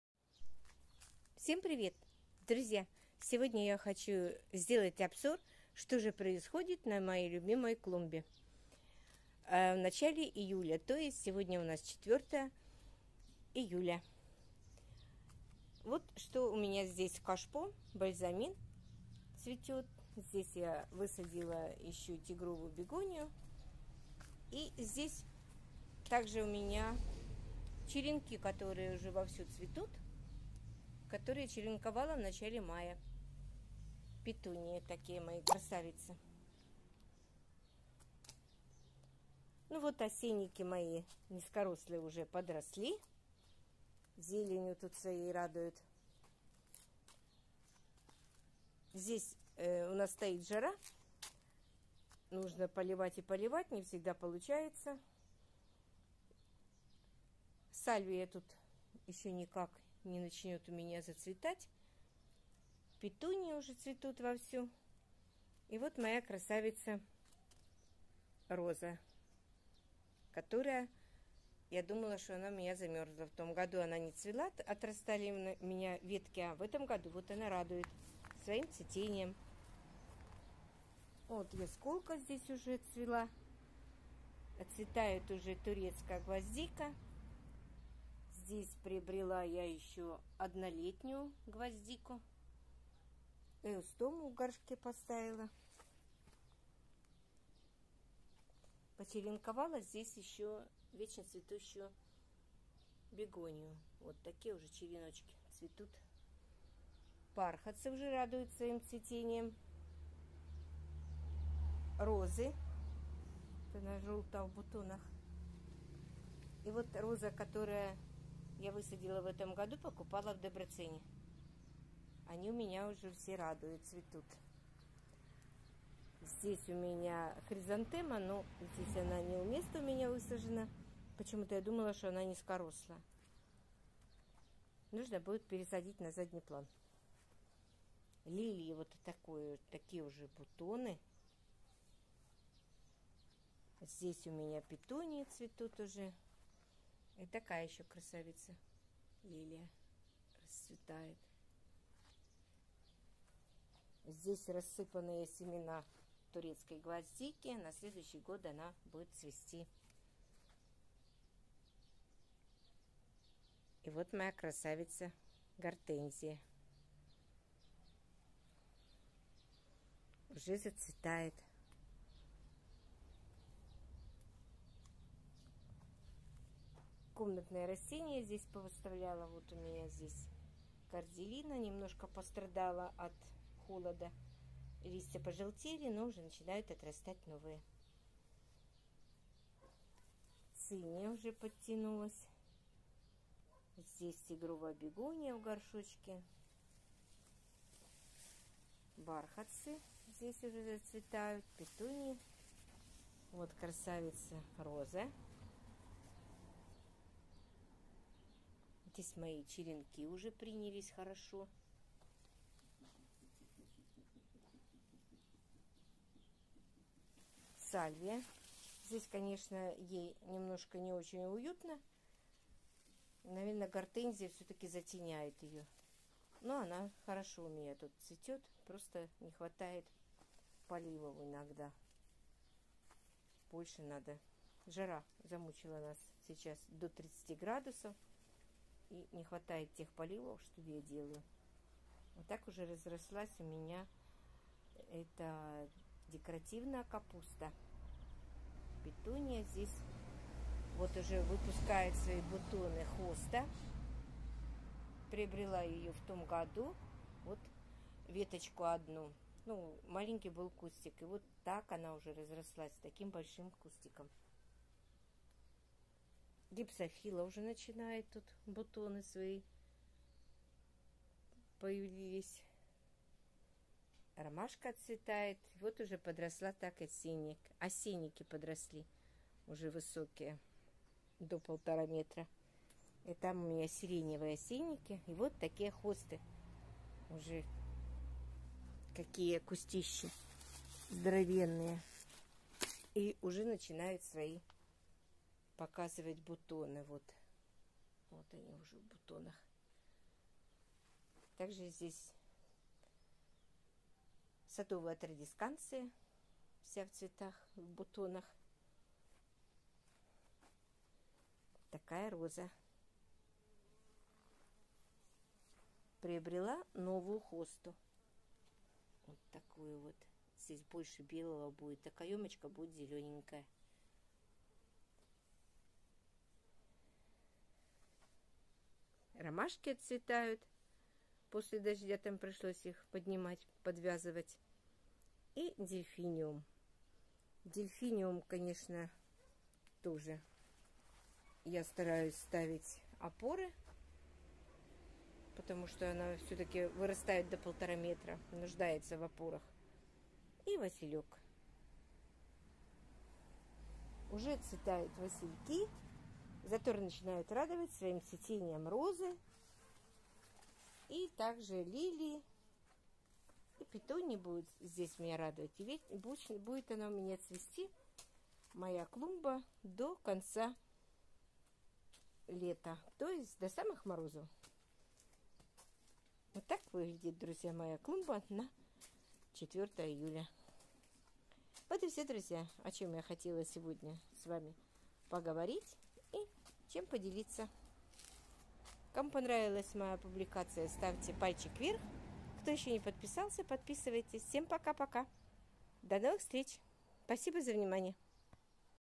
Обзор цветущей клумбы с весны до заморозков в июле месяце.
Друзья, хочу сделать обзор своей любимой клумбы в начале июля, которая начинает цвести с весны до самых морозов что-то отцветает а что-то ещё только начинает зацветать и так по очереди за самых морозов Но есть такие цветочки которые цветут всё лето и до морозов как например петунии, сальвия и бархатцы.
поэтому я всегда их выращиваю рассадным способом чтобы высадить их пораньше эти полюбившие  однолетние цветы.